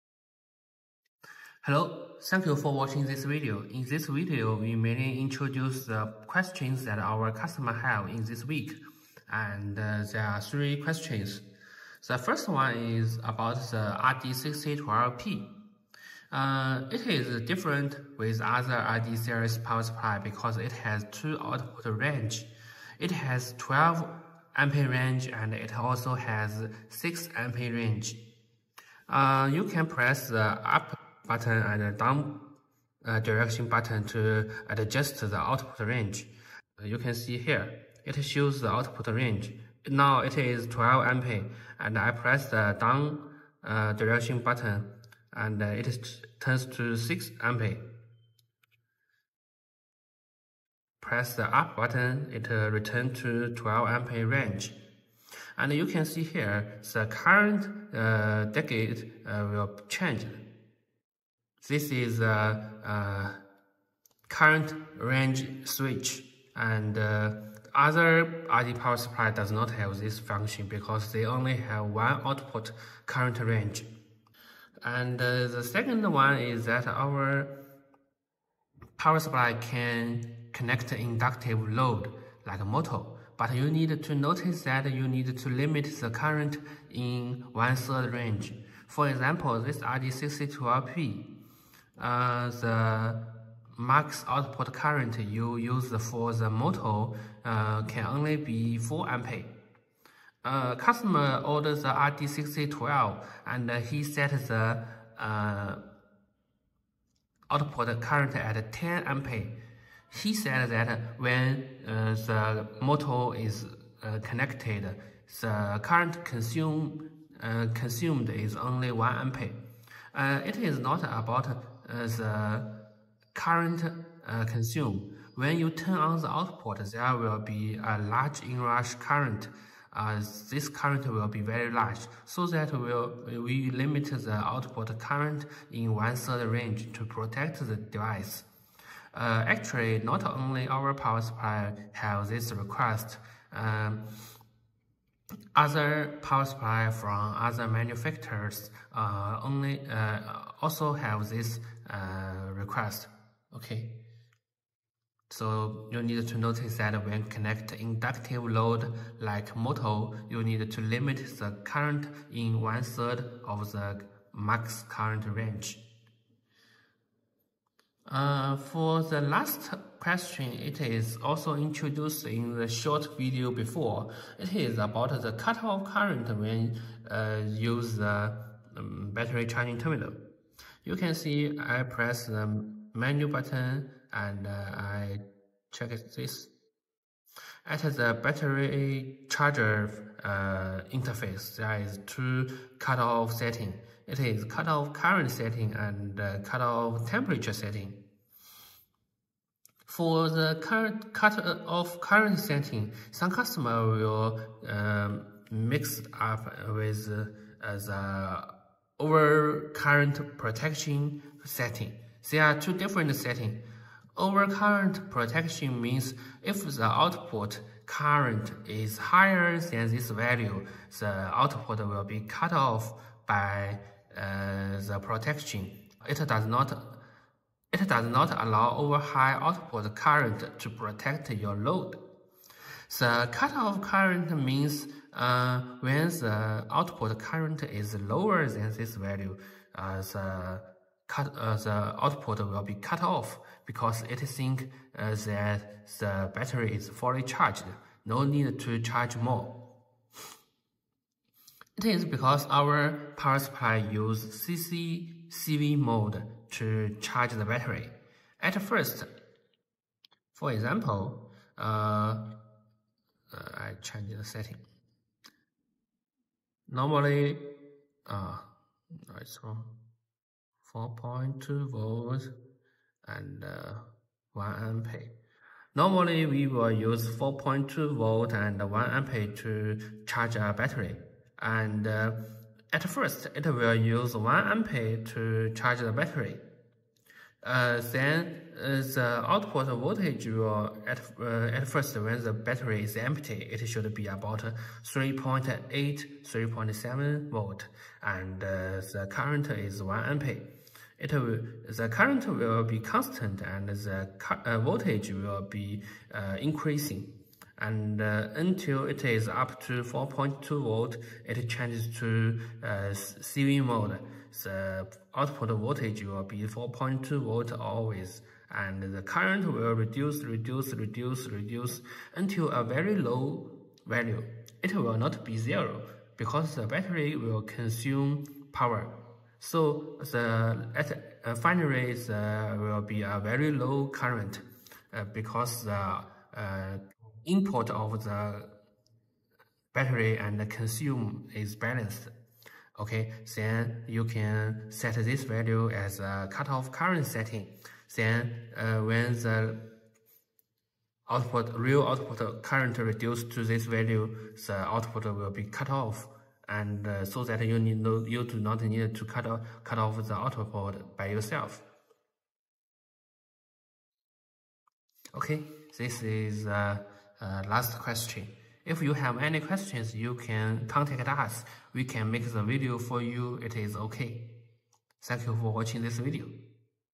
RD6012P 6A 12A switch, motor current limit, cutoff current setting
hello, in this video we mainly talk about 3 questions
1. some customers said that RD6012P cannot output over 6A, because it is set to 6A range, you should set it back to 12A
2, when connect inductive load (such as motor), the max working voltage and current is 1/3 of full range. don't exceed it.
3. cutoff current is not OCP, it is the value to cutoff the battery charging, because the battery is fully charged, no need to charge anymore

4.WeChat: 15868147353